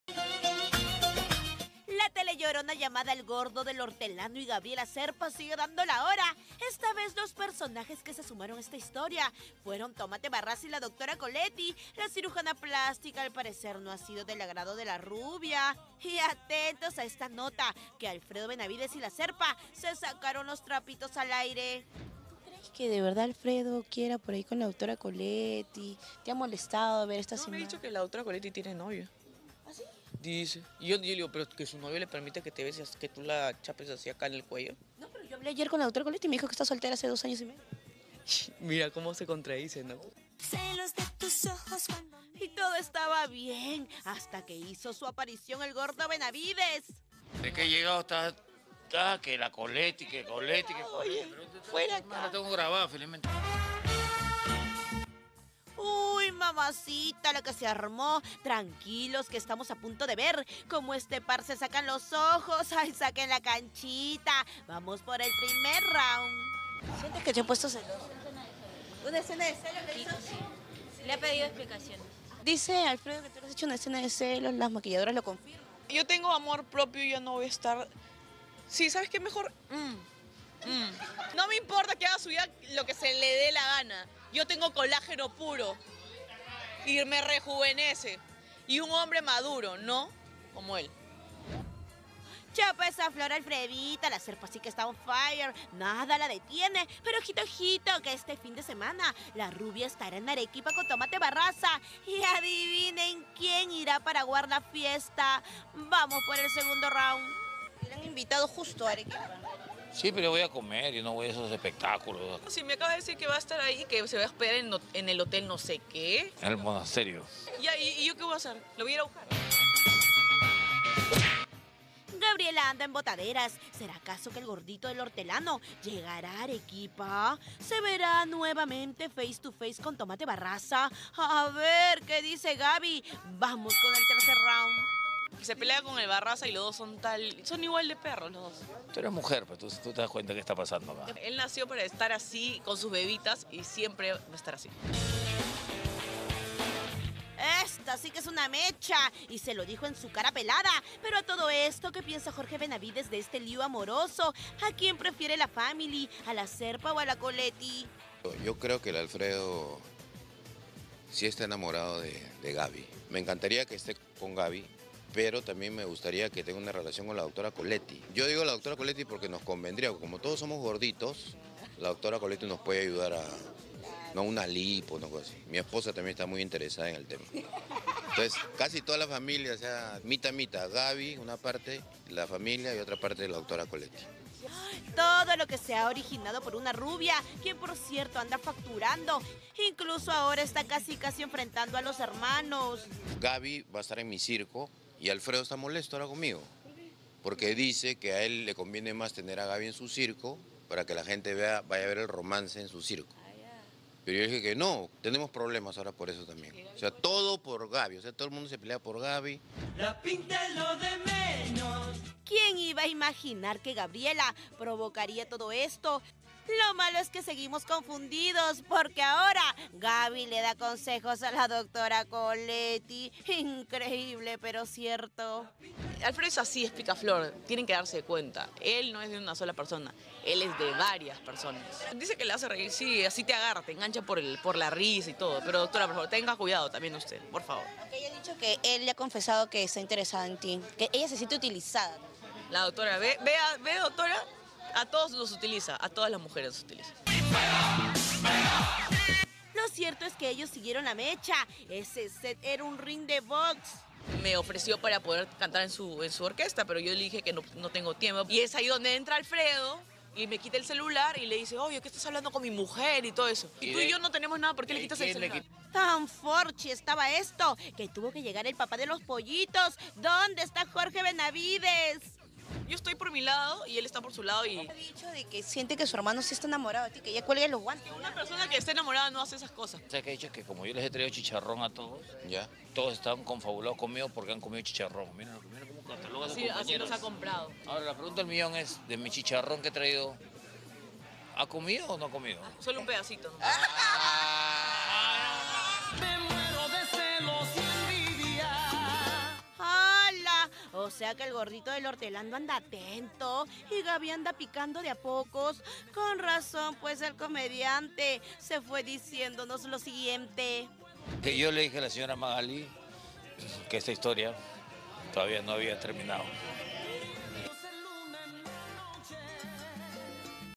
Gabriela Serpa y Alfredo Benavides se encuentran y se dicen sus verdades
La actriz y el cómico se 'sacaron los trapitos'.